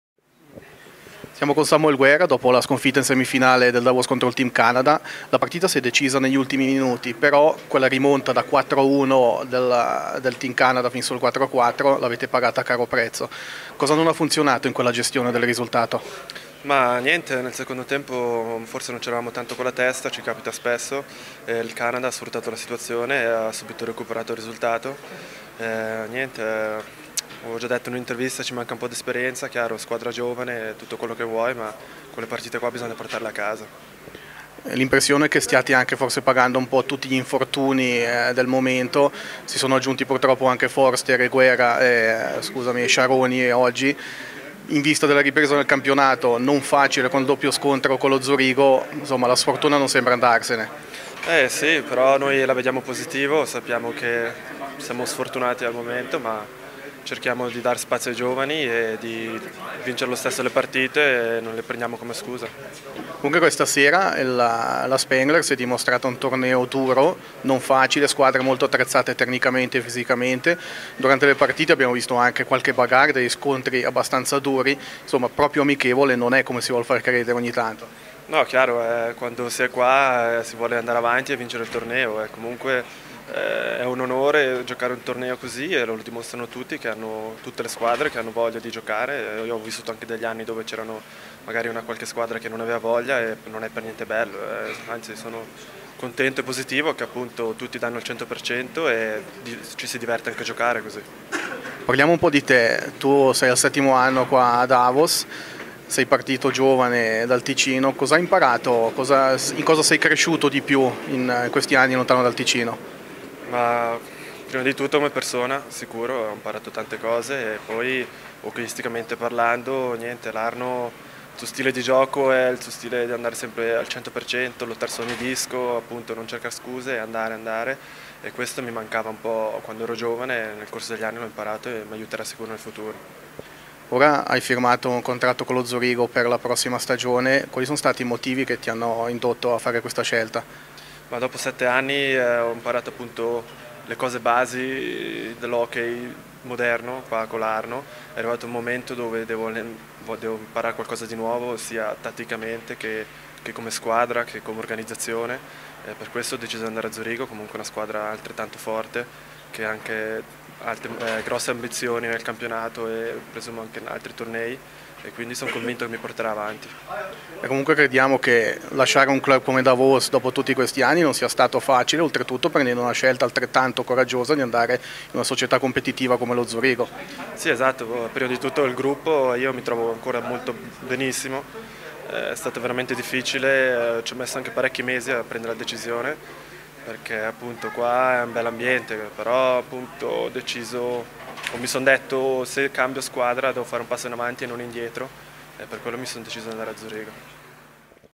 Intervista a Samuel Guerra - HC Davos - 30.12.2015
Intervista al difensore del Davos, Samuel Guerra, al termine della partita contro il Team Canada.